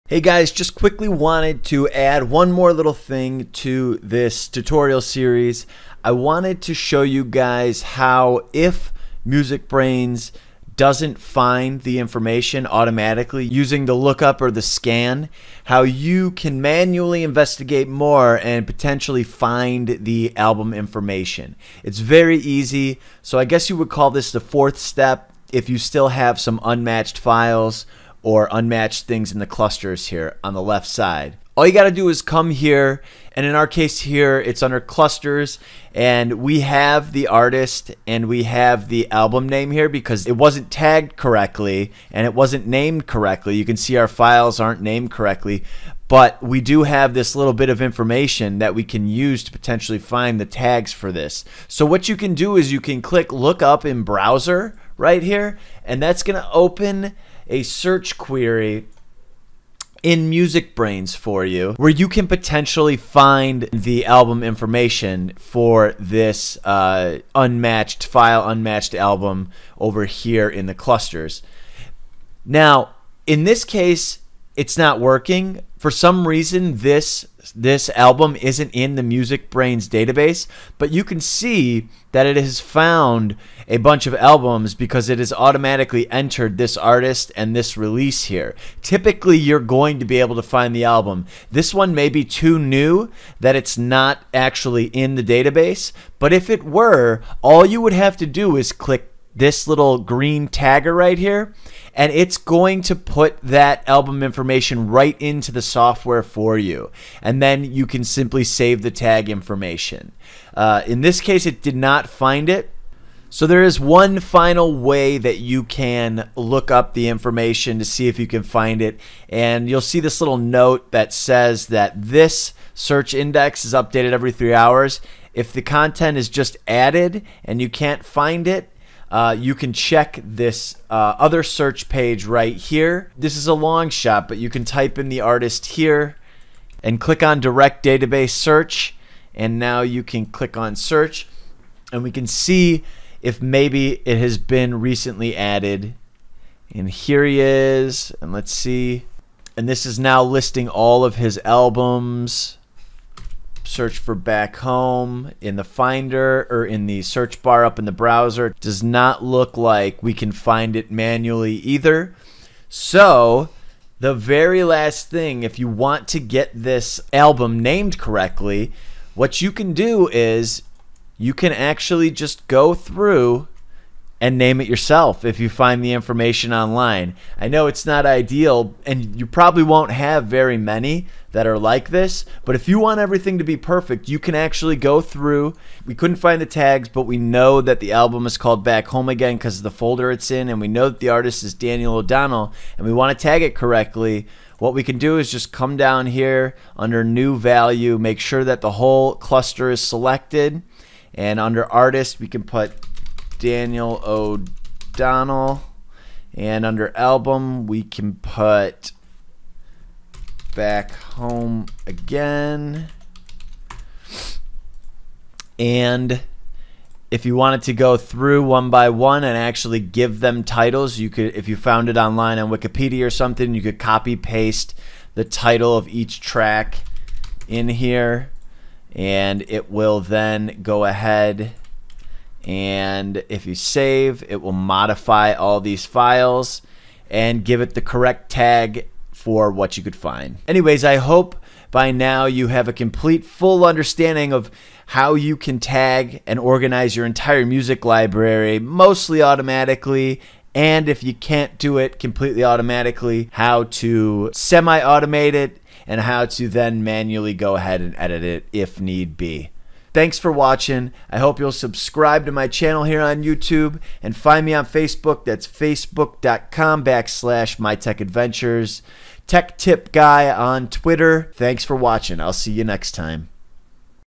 How to Automatically Tag and Organize a Music Library PART 2
This is part 2 of short tutorial series that explains how you can tag, rename and organize your music library using the FREE MusicBrainz Picard. In it you will learn how to find album/artist/tag information when Musicbrainz Picard can not find the mp3 tag information (song title, album title, album year, artist, etc...) automatically using the method laid out in PART 1.